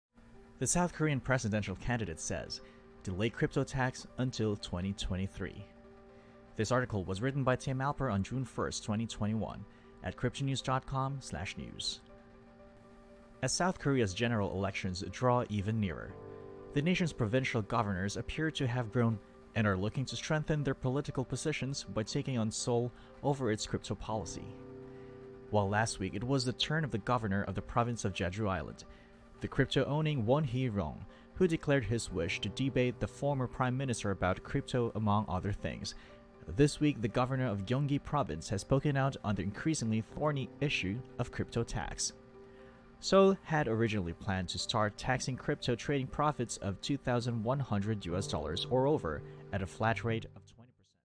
South Korean Presidential Candidate: Delay Crypto Tax Until 2023
Coinbase is one of the best, safest and easiest places to buy and sell cryptocurrency.

Written By: Tim Alper

As South Korea’s general elections draw ever nearer, the nation’s provincial governors appear to have grown bolder – and are looking to strengthen their political positions by taking on Seoul over its crypto policy.

While last week it was the turn of the Governor of the province of Jeju Island, the crypto-owning Won Hee-ryong, who declared his wish to debate the former Prime Minister about crypto among other things, this week the Governor of Gyeonggi Province has spoken out on the increasingly thorny issue of crypto tax.

Seoul had originally planned to start taxing crypto trading profits of USD 2,100 or over at a flat rate of 20% from October this year, but after pleas from exchanges decided to delay the launch of taxes until January 1, 2022. However, the plans have been the subject of bitter criticism from mainly younger South Koreans – who happen to make up the bulk of the ruling Democratic Party’s support.

This criticism has led many in the ruling party to hint that a tax U-turn is not impossible, even at this late stage. And per Hanguk Kyungjae and WowTV, Lee Jae-myung, the Gyeonggi Province Governor and one of the two leading candidates for the Democratic Party presidential candidate nomination, has called for a one-year delay to the launch of the tax.

Lee Jae-myung, the former mayor of Seongnam, is around 5% points behind his major challenger for the nomination, but media reports claim the gap is closing fast.

He stated that delaying the tax by a year would allow the new levy to launch in sync with a revised stock trading tax law that is set to promulgate in January 2023.

Under Lee Jae-myung’s governorship, a number of local stablecoin projects in the province – which surrounds Seoul, is the nation’s richest and is also home to most of the country’s biggest tech and electronics giants – have expanded rapidly.

Meanwhile, the Jeju Governor Won, reported Yonhap last week, also fancies a tilt at the presidency – and called for a “crypto debate” with Lee Nak-yon, the former Prime Minister. Lee Nak-yon is now the leader of the Democratic Party and also one of the crypto sector’s fiercest critics.

Won has gone on the record as stating that he bought some USD 900 worth of bitcoin (BTC) and other coins in a bid to better understand the viewpoints of the crypto-keen youth – and was challenged by the former PM on the sagacity of his decision.

Won claimed in retaliation that Lee Nak-yon and the government were guilty of “neglecting” the crypto sector and knew “nothing about the crypto trading market.”